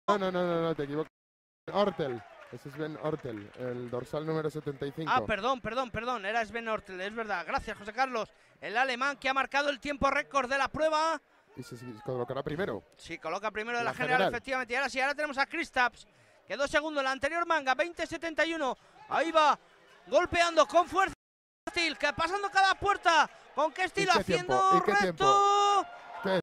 Kristaps Zvejnieks Inline Alpine World Cup Villablino, Spain 2nd Run
Kristaps Zvejnieks in 2nd Run during Inline Alpine World Cup in Villablino, Spain. Finished in 2nd place by 0,01 seconds from Marco Walz (GER). Video from xamascada.com.